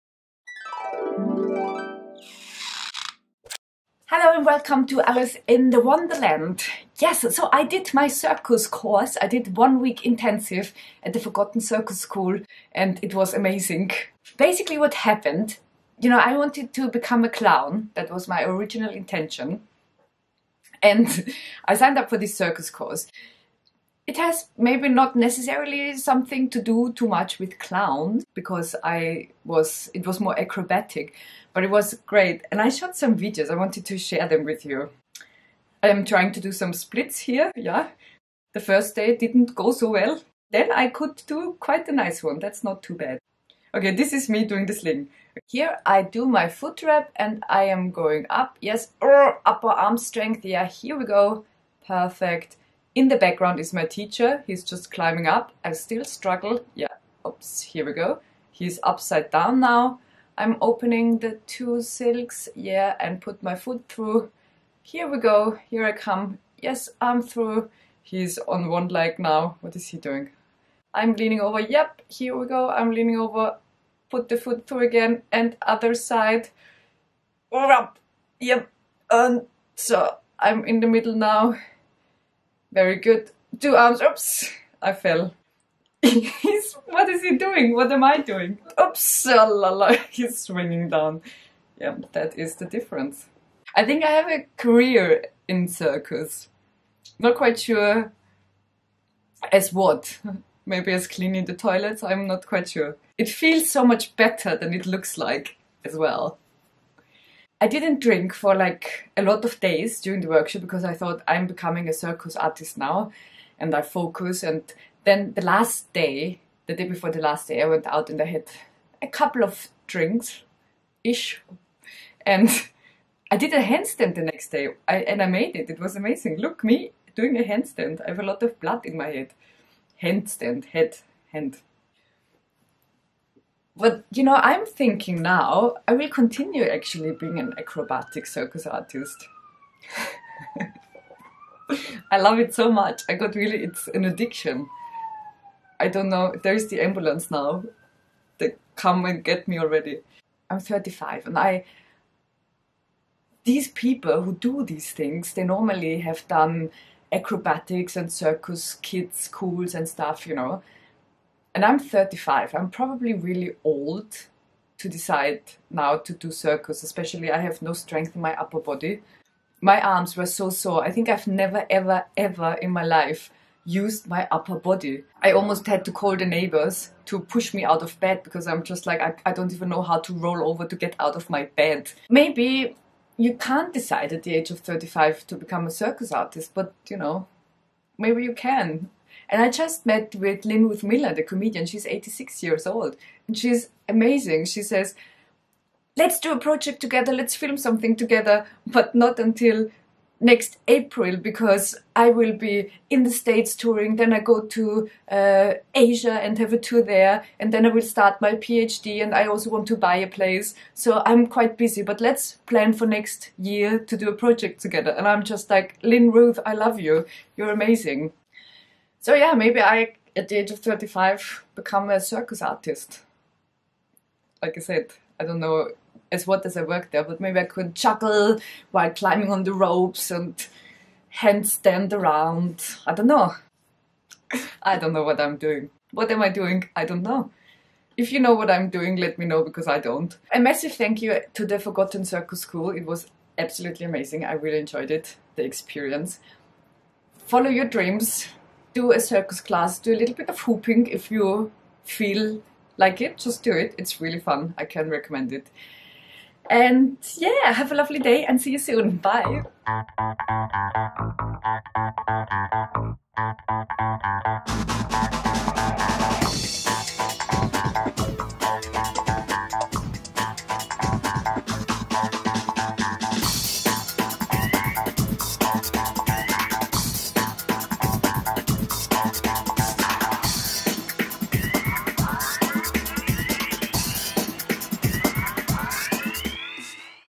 Cirque du Alice
I did a week at the Forgotten Circus School... This is me "climbing" the ropes and swinging the hoops! Definitely a week to remember :)

Alice in the Wonderland - Comedy v-log